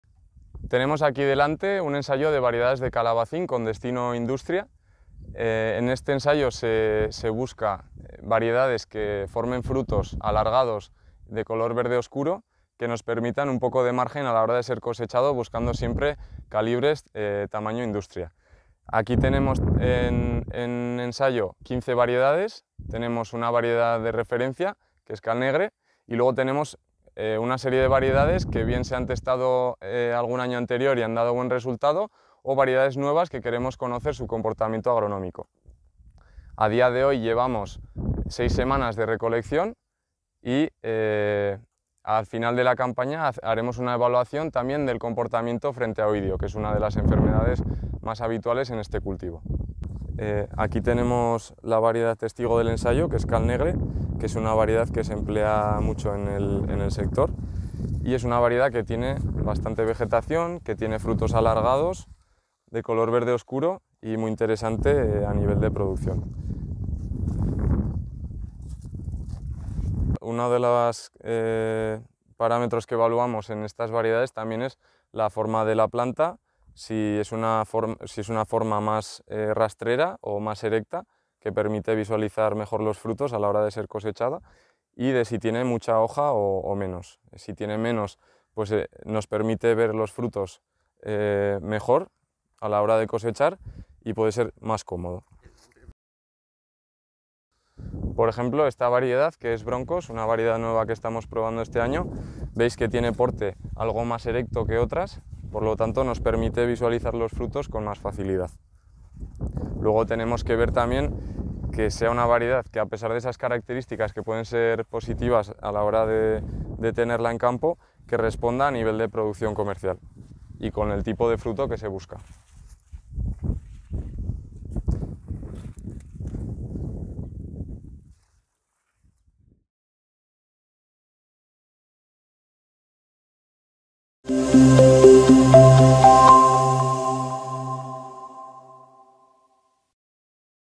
Ensayo de variedades de calabacín
Ensayo experimental de variedades de calabacín

Finca Experimental de Cadreita (Navarra)

Iñigo Arozarena, técnico de Experimentación de INTIA y especialista en cultivos hortícolas.

Proyecto LIFE NAdapta, estrategia de adaptación al Cambio Climático del Gobierno de Navarra